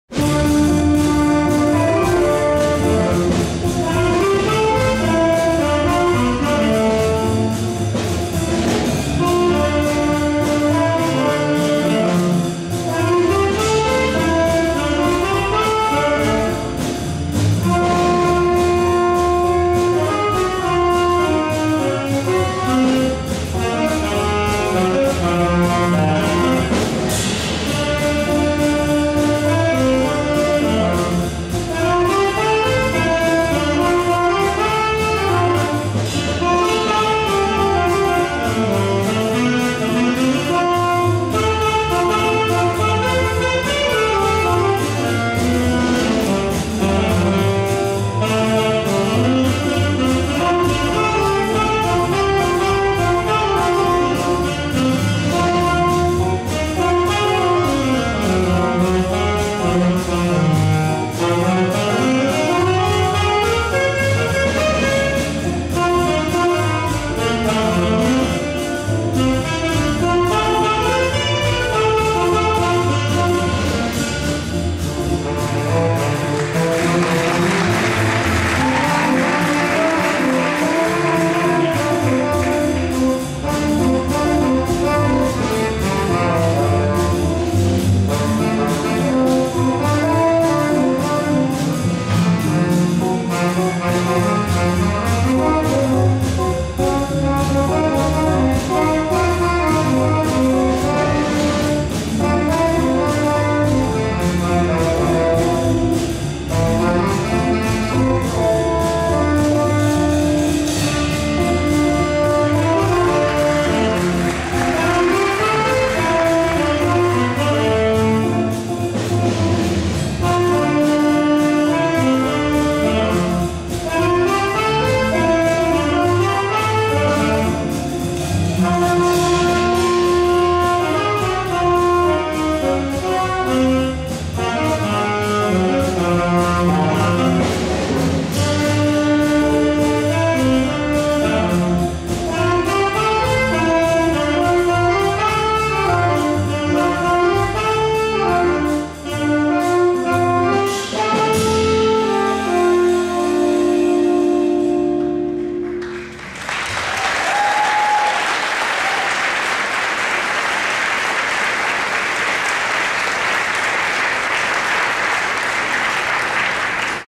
Rural Jazz
The final performance of the Grade 12 Charlottetown Rural High School Jazz Ensemble May 17, 2010